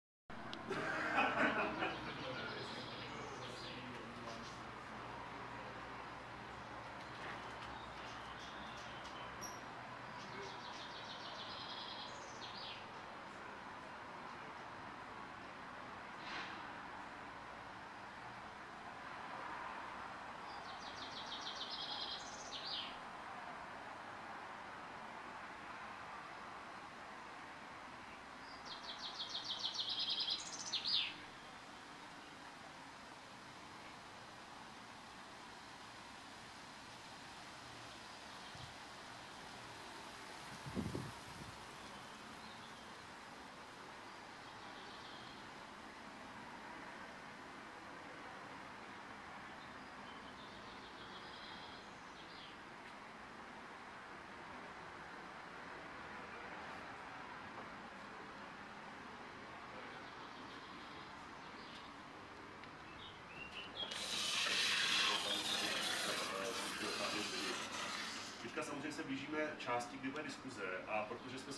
Procházka po molu terasy Hospůdky Ohře
Během pirátské konference jen vítr ševelil listy